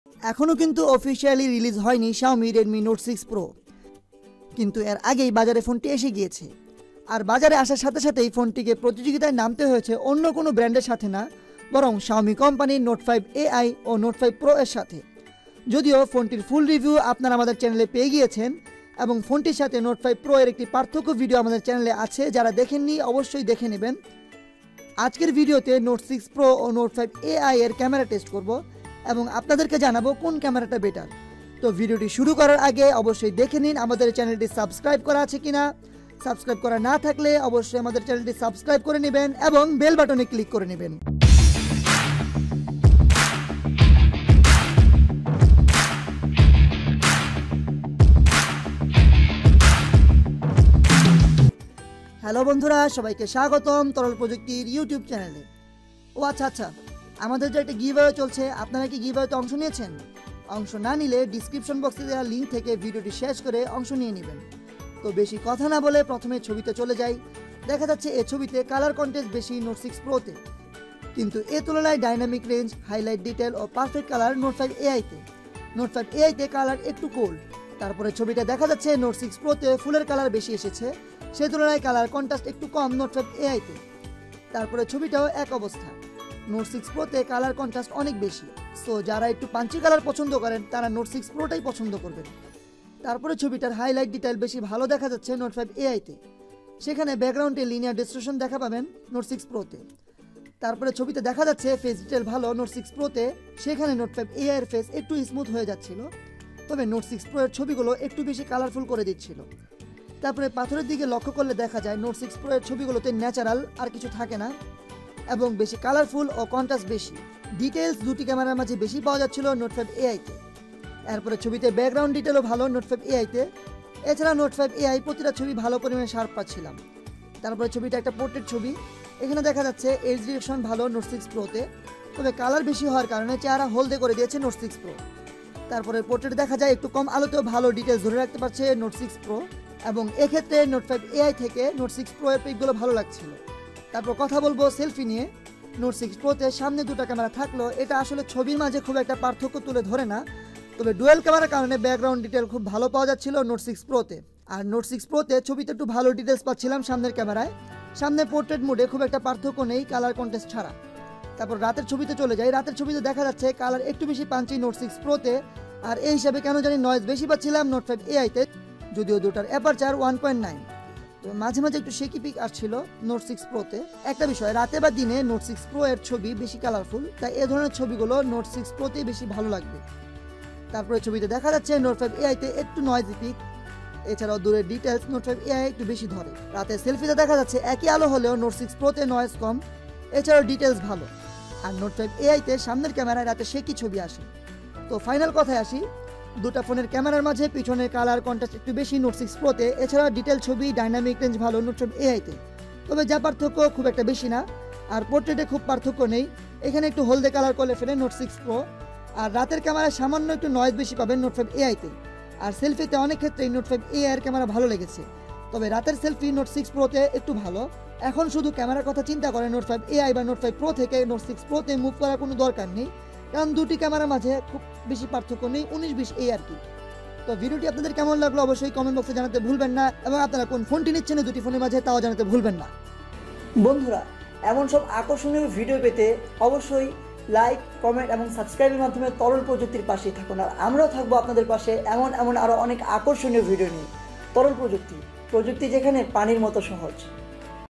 Xiaomi Redmi Note 6 Pro vs Redmi Note 5 Ai Camera Comparison with Pictures (Bangla)
In this video we discussed about the camera test, comparison of xiaomi redmi note 6 pro and redmi note 5 ai china version. They are the best price to performance ratio smartphones from xiaomi. Here we compared pictures from both phones in daylight & night time.

This content is original and copyright to TorolProjukti™. Legal action will be taken against those who violate the copyright.